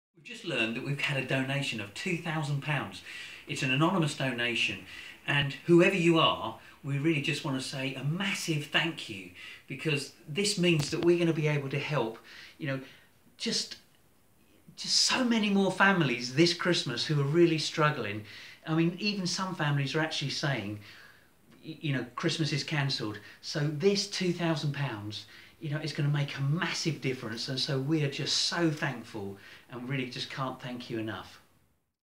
A Donation of £2000! - Thank You So Much
"Thank you to the anonymous donor who donated £2000 via the Facebook link, this will really make a difference this Christmas to families struggling to make ends meet!  It is very much appreciated" - Henry Wilson, Projects Director.